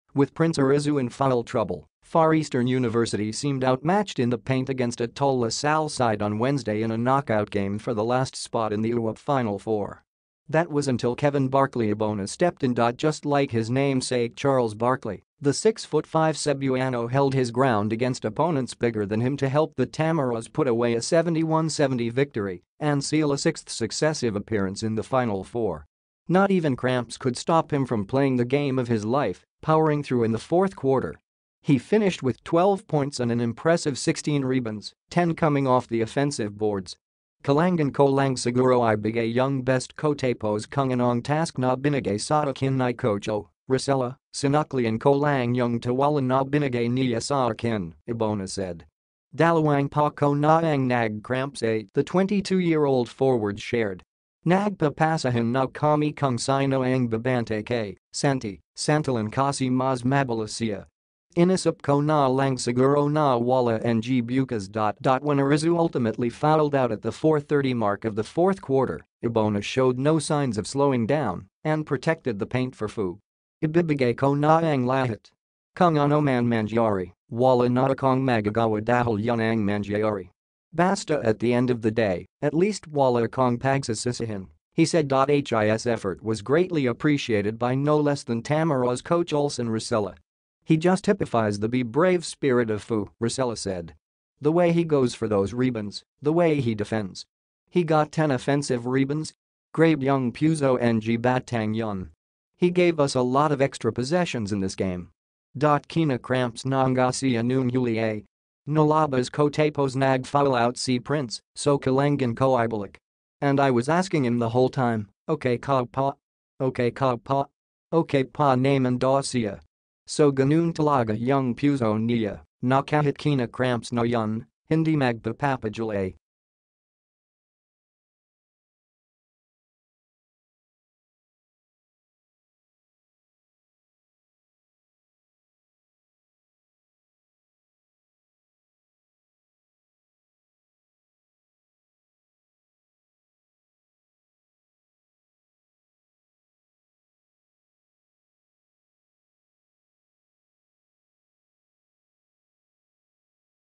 Just like namesake, Barkley Ebona crashes boards relentlessly for FEU
WITH Prince Orizu in foul trouble, Far Eastern University seemed outmatched in the paint against a tall La Salle side on Wednesday in a knockout game for the last spot in the UAAP Final Four.

That was until Kevin Barkley Ebona stepped in.

Just like his namesake Charles Barkley, the 6-foot-5 Cebuano held his ground against opponents bigger than him to help the Tamaraws put away a 71-70 victory and seal a sixth successive appearance in the Final Four.

Not even cramps could stop him from playing the game of his life, powering through in the fourth quarter. He finished with 12 points and an impressive 16 rebounds, 10 coming off the offensive boards.

"Kailangan ko lang siguro ibigay yung best ko tapos kung anong task na binigay sa akin ni Coach O (Racela), sinuklian ko lang yung tiwala na binigay niya sa akin," Ebona said.

"Dalawang paa ko na ang nag-cramps eh," the 22-year-old forward shared. "Nagpapasahan na kami kung sino ang babantay kay (Santi) Santillan kasi mas mabilis siya. Inisip ko na lang siguro na wala ng bukas."

When Orizu ultimately fouled out at the 4:30 mark of the fourth quarter, Ebona showed no signs of slowing down and protected the paint for FEU.

"Ibibigay ko na ang lahat. Kung ano man mangyari, wala na akong magagawa dahil yun ang mangyayari. Basta at the end of the day, at least wala akong pagsisisihan," he said.

His effort was greatly appreciated by no less than Tamaraws coach Olsen Racela.

"He just typifies the Be Brave spirit of FEU," Racela said. "The way he goes for those rebounds, the way he defends. He got 10 offensive rebounds? Grabe yung puso ng batang yun. He gave us a lot of extra possessions in this game."

"Kina-cramps na nga siya noong huli eh. Nilabas ko tapos nag-foul out si Prince, so kailangan ko ibalik. And I was asking him the whole time, 'Okay ka pa? Okay ka pa?' Okay pa naman daw siya.

"So ganoon talaga yung puso niya, na kahit kina-cramps na yun, hindi magpapapigil eh."